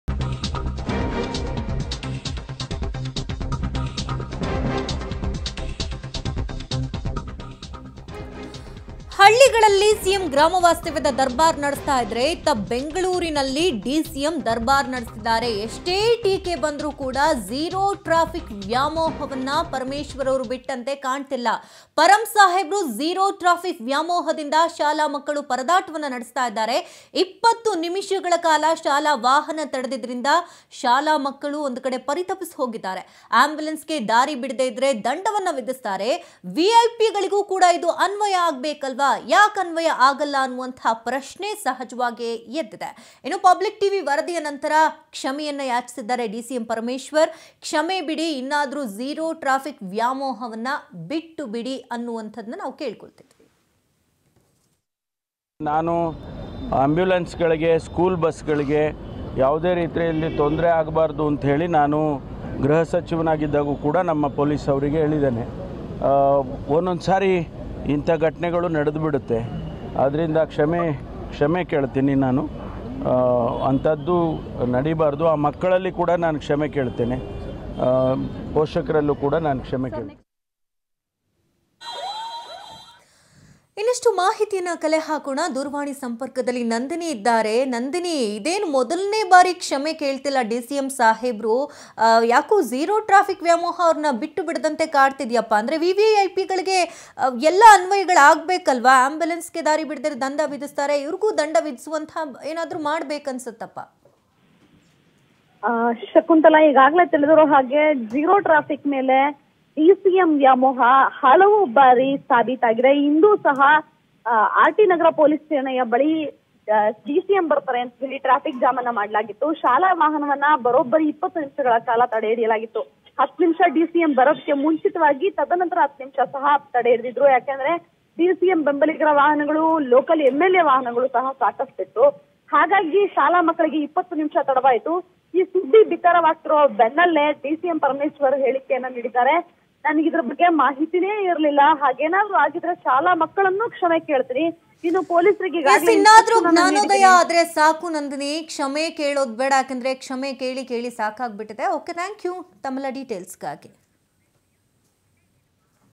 'Zero Traffic' Parameshwar Apologizes To School Students For Causing Traffic Jam In RT Nagar
DCM Parameshwar apologized to school students for causing a traffic jam in RT Nagar. Parameshwar had arrived to inspect road works in RT Nagar. Massive traffic jam happened as the vehicular movement was blocked to ensure zero traffic for the deputy CM.